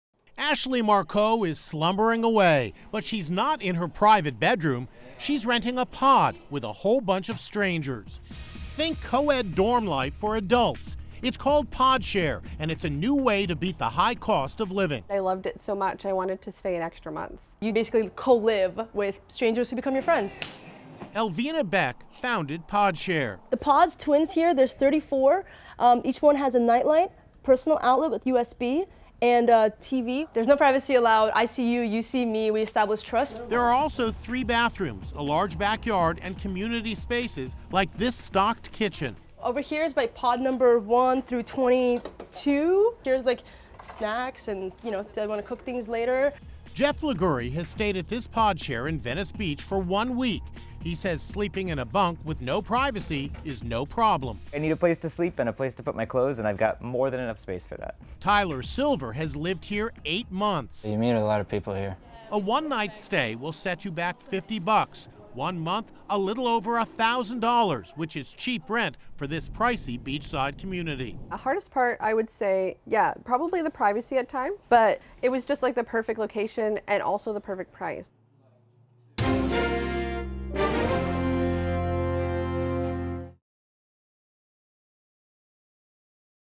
Living in This Pod Will Cost You $1,000 a Month
Podshare is a new way to beat the high cost of living and Inside Edition met with some residents in Los Angeles who are calling the capsules home. “I loved it so much, I wanted to stay and extra month,” resident Ashley Marcoux told Inside Edition. Elvina Beck founded Podshare and told Inside Edition that “there is no privacy allowed. You see me. I see you. We establish trust.” Podshare is sort of like a co-ed dorm but for adults.